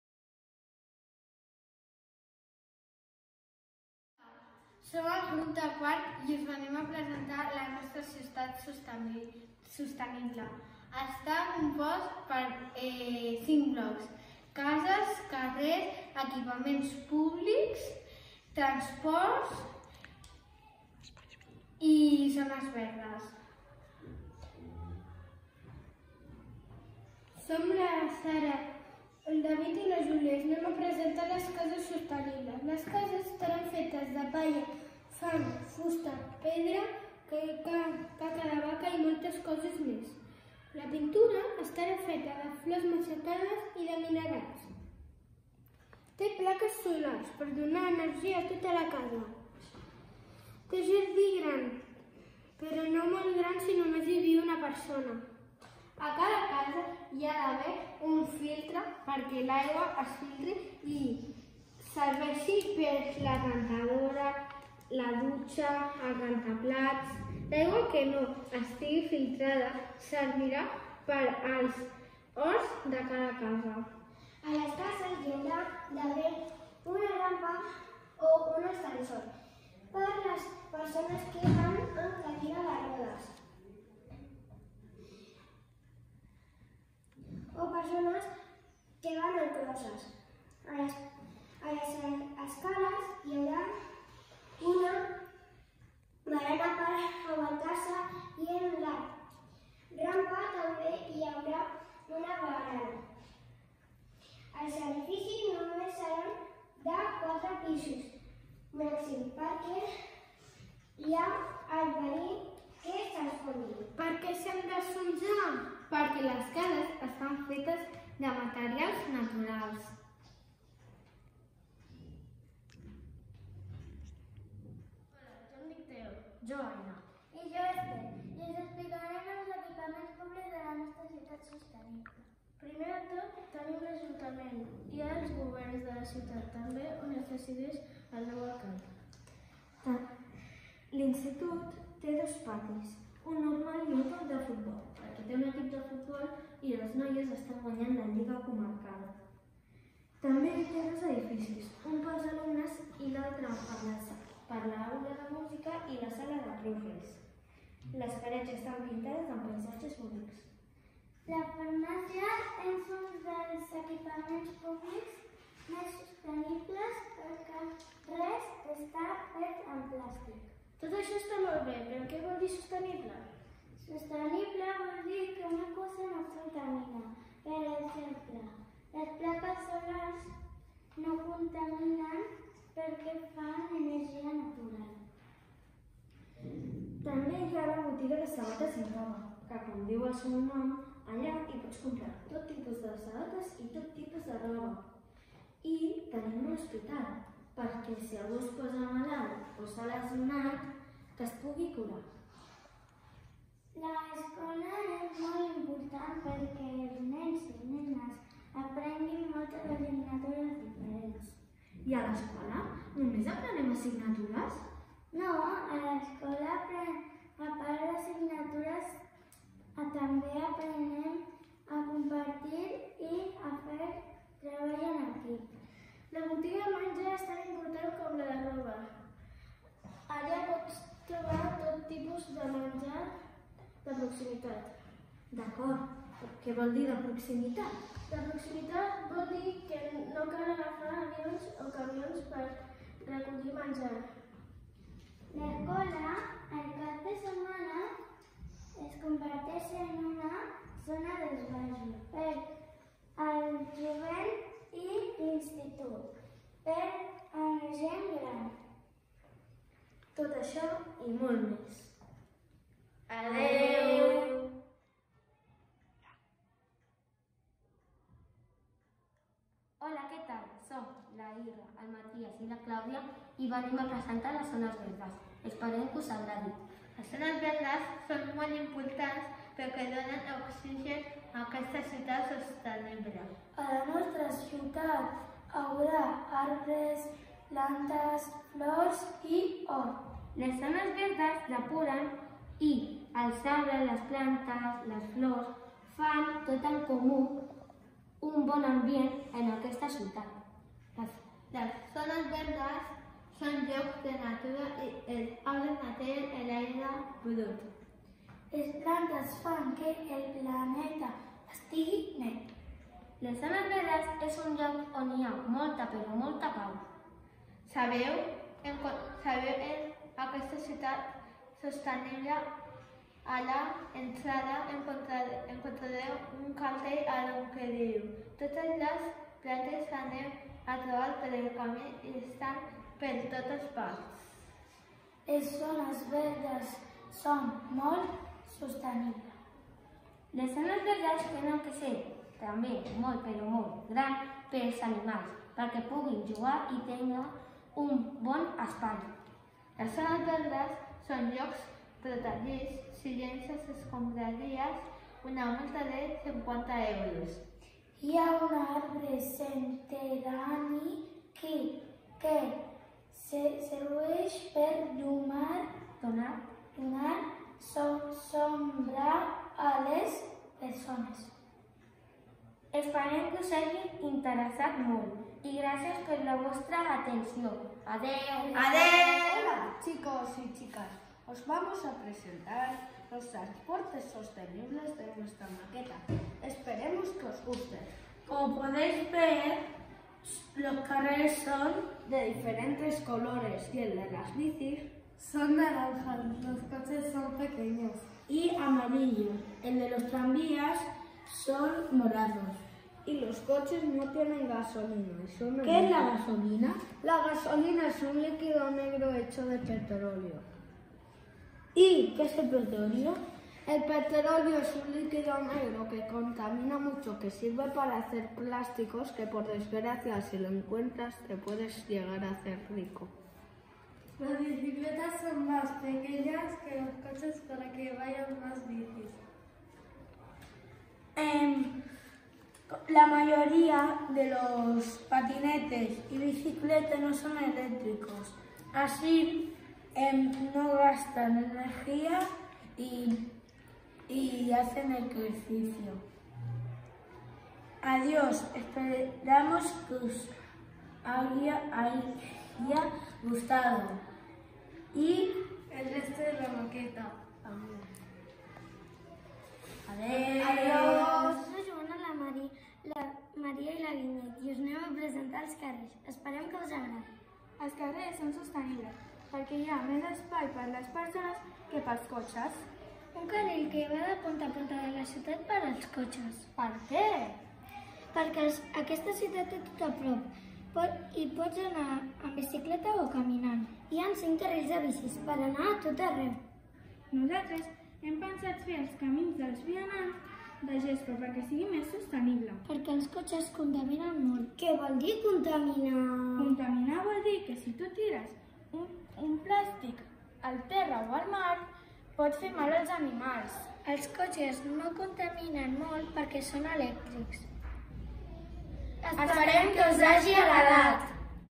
La Ciutat Sostenible de 4t
Els nens i nenes de 4t hem dissenyat la maqueta de la Ciutat que volem.